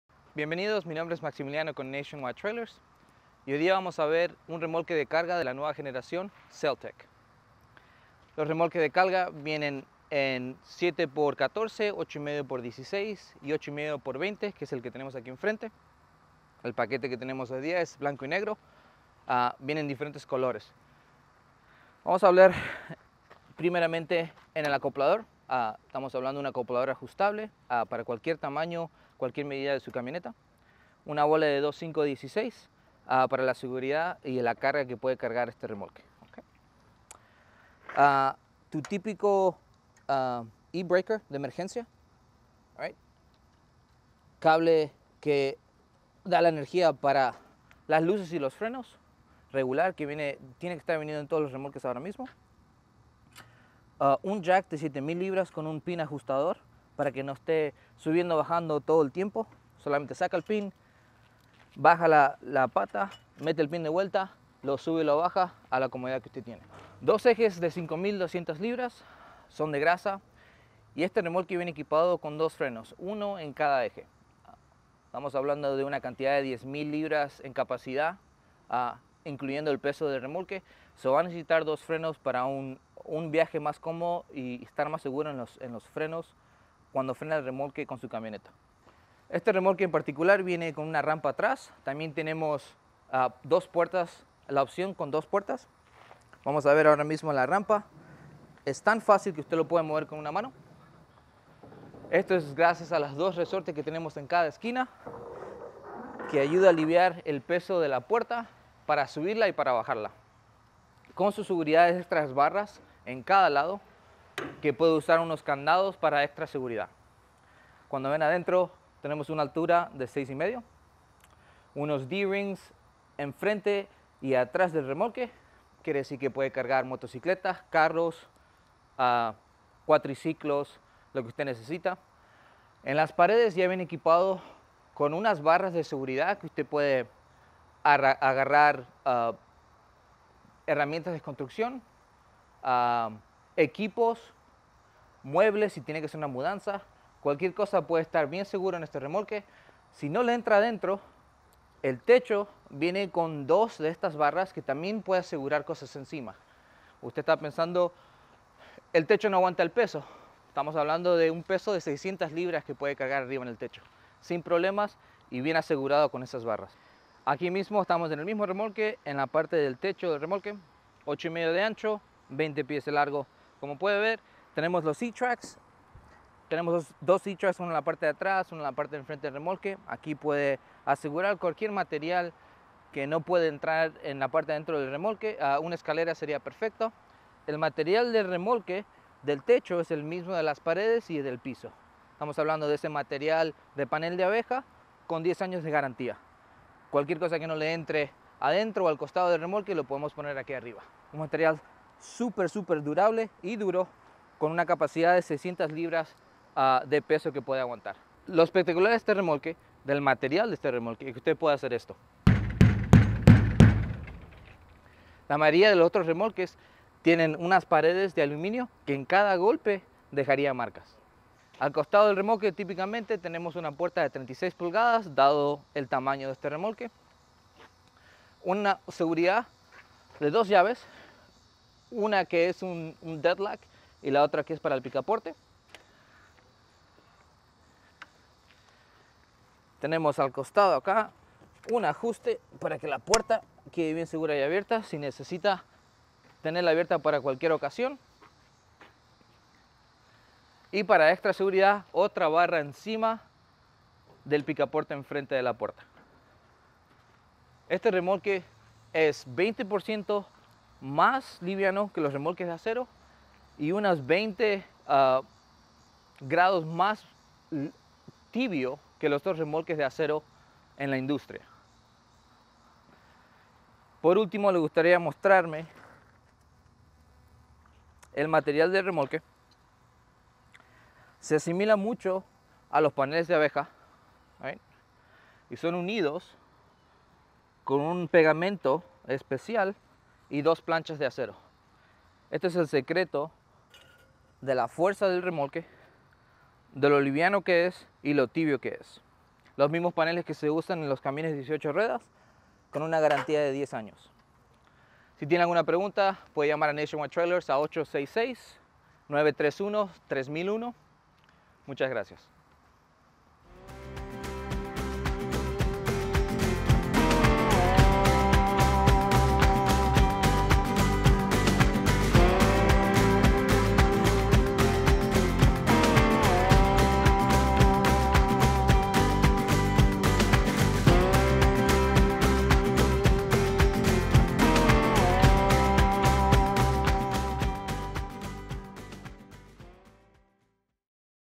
Remolque CellTech de 8.5'x20' Para Caminar
Escuche como el experto en tráileres Max Estrella habla sobre el revolucionario tráiler de CellTech.

CellTech ha desarrollado una tecnología de sándwich de metal liviano patentada con una tremenda relación rigidez-peso.

El diseño sándwich de CellTech es una estructura corrugada bidireccional de flujo de aire abierto. Los paneles CellTech están diseñados para lograr características creativas óptimas para los objetivos de resistencia/rigidez a la compresión y peso.

Los paneles de CellTech son muy diferentes de los paneles con núcleo de espuma, por lo que la tecnología es superior en la entrega de atributos estructurales que no se deterioran con el tiempo.

El diseño patentado de CellTech ayuda a que el acero sea más liviano y el aluminio más resistente a través de su diseño de sándwich multicapa.

El resultado es un material sin ondas y sin remaches que hace que este tráiler sea perfecto para gráficos.

En Houston, comuníquese con Max al o pase por su distribuidor de Nationwide Trailer más cercano y recoja uno hoy.